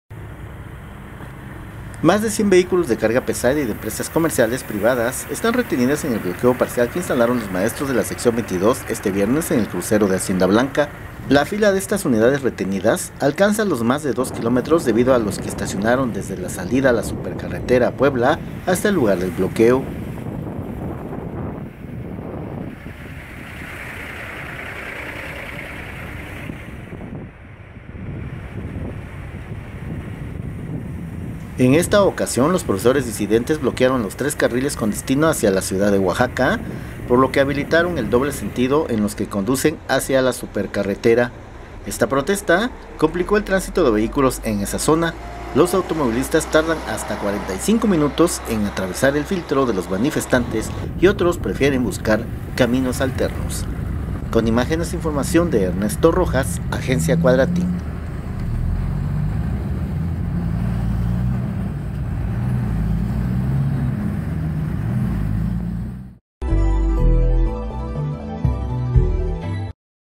Retienen maestros más de 100 vehículos en Hacienda Blanca
OAXACA, Oax., 22 de julio de 2016.- Más de 100 vehículos de carga pesada y de empresas comerciales privadas están retenidos en el bloqueo parcial que instalaron maestros de la Sección 22 este viernes en el crucero de Hacienda Blanca.
La fila de estas unidades retenidas alcanza los más de 2 kilómetros, debido a que los estacionaron desde la salida a la supercarretera a Puebla hasta el lugar del bloqueo.
En esta ocasión, los profesores disidentes bloquearon los tres carriles con destino hacia la ciudad de Oaxaca, por lo que habilitaron el doble sentido en los que conducen hacia la supercarretera.